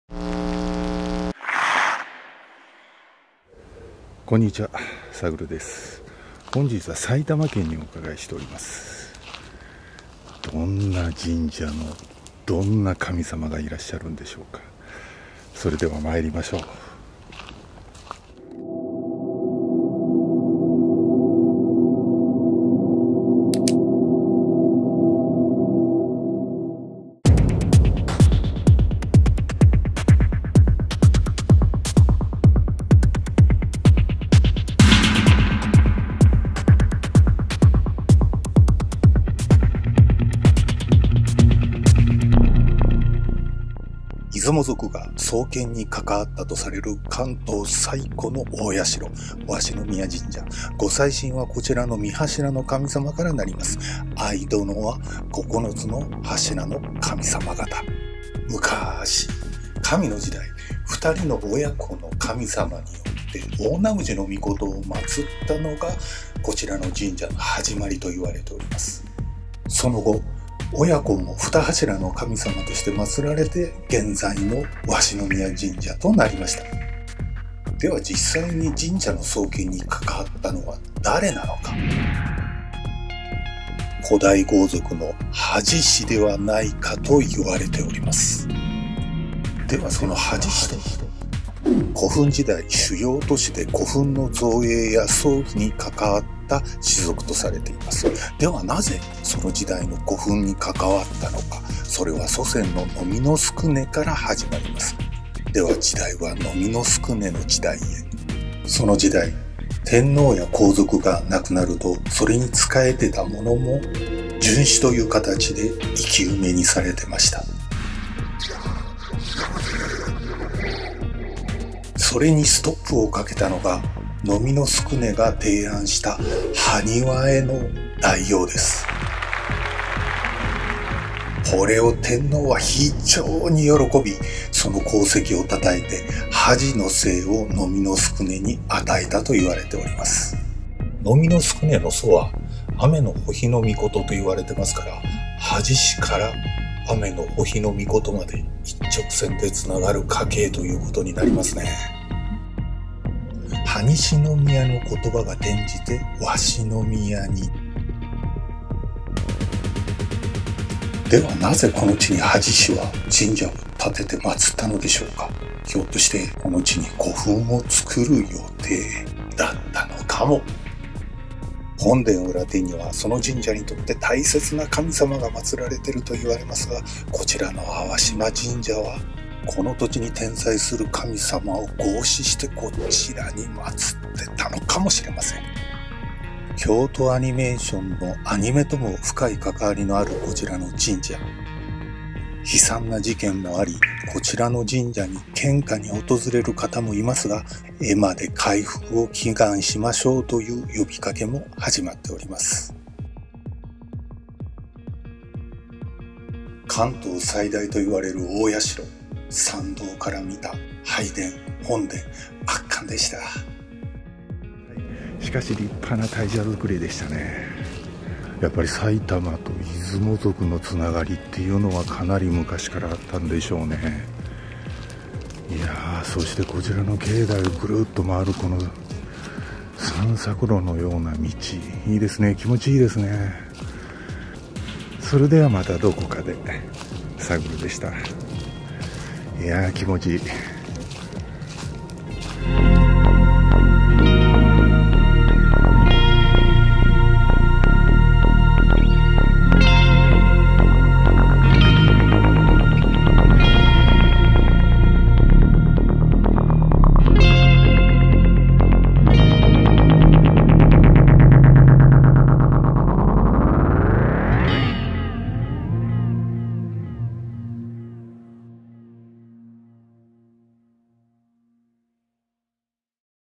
関東最古の大社 鷲宮神社 ミステリアス紀行#71
鷲宮神社#71 出雲族の草創に係る関東最古の大社
またもや埼玉と出雲関係深い神社です 参道から見えてくる横からの本殿は必見です１